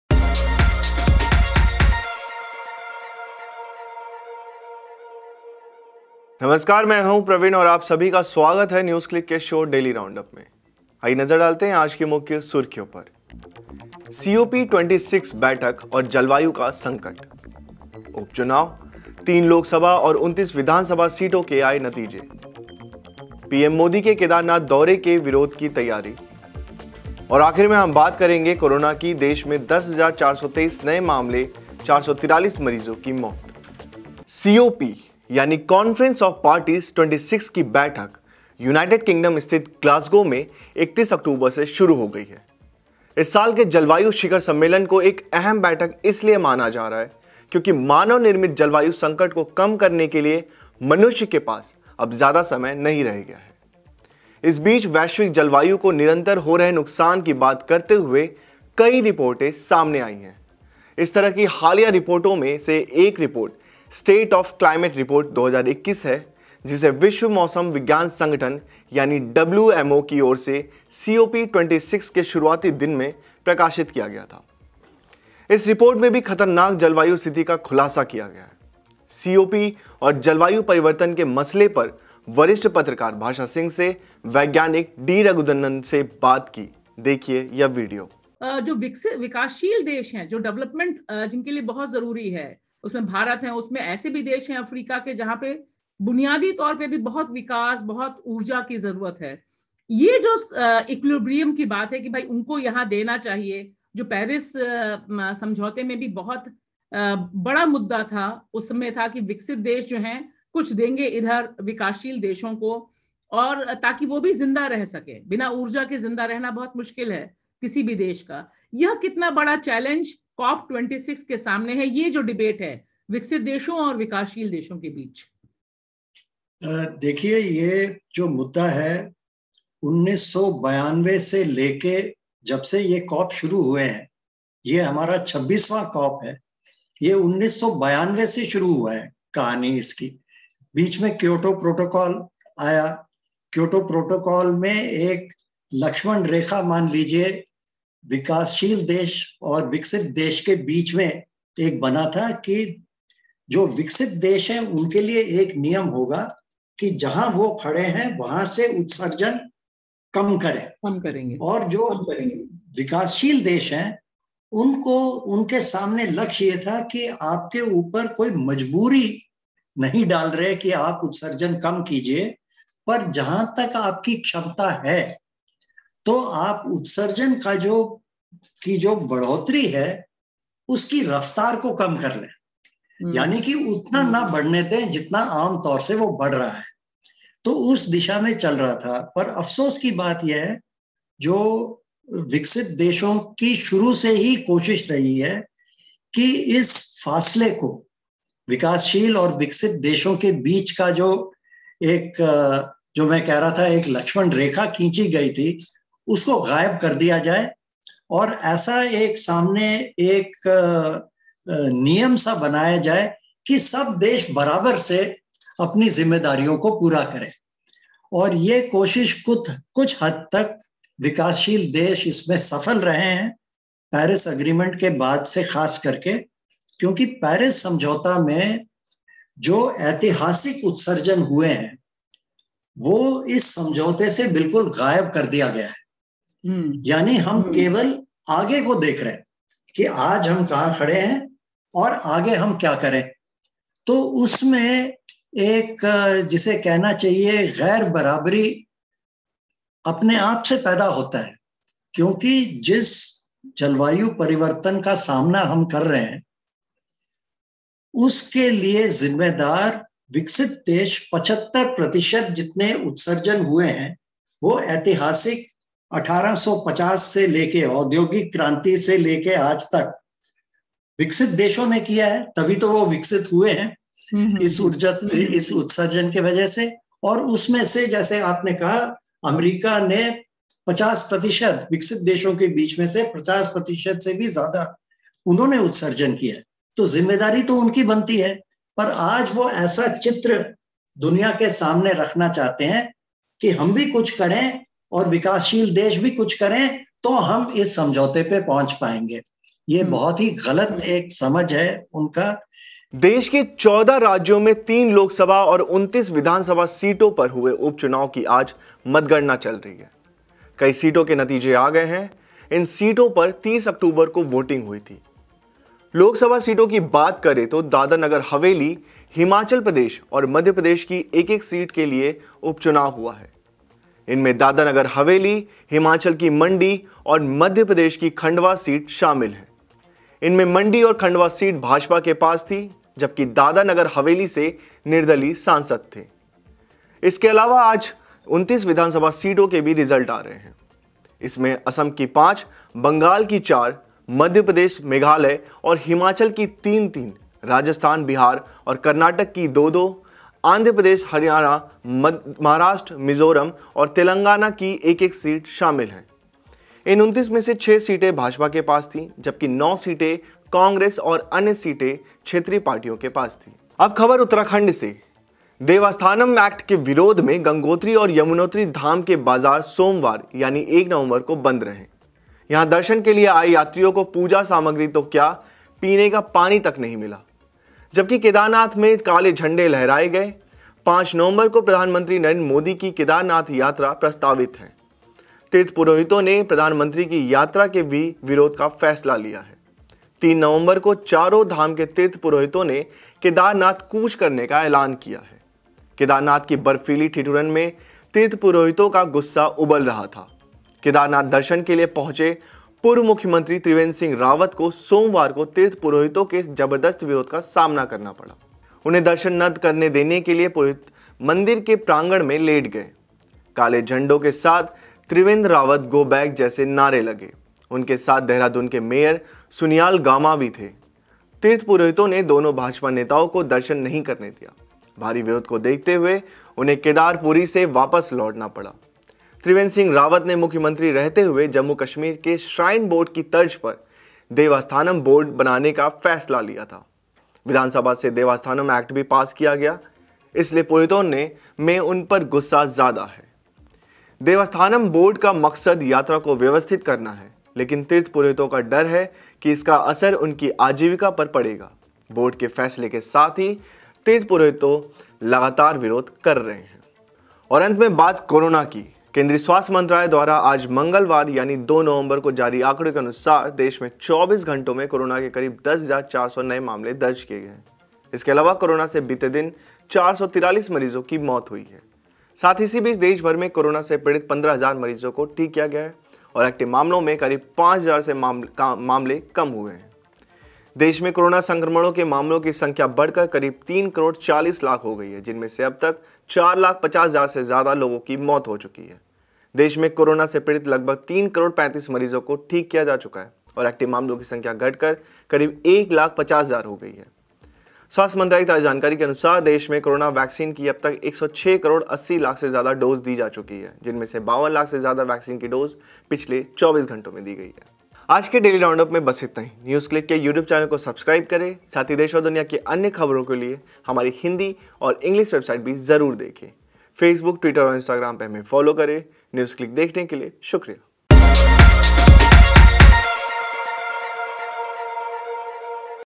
COP26 बैठक, उपचुनाव नतीजे और अन्य ख़बरें
न्यूज़क्लिक के डेली राउंडअप में आज हम बात करेंगे COP26 बैठक, उपचुनावों के नतीजे और अन्य ख़बरों के बारे में।